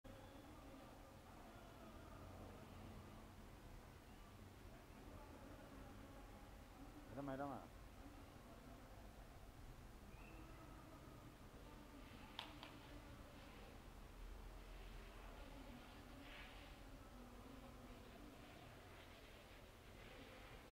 YSR Kutumbam Funtion at Bengaluru on 23-12-2018
On Occasion of YS Jagan Birthday Celebrations and Discussion for Stratergy to next Elections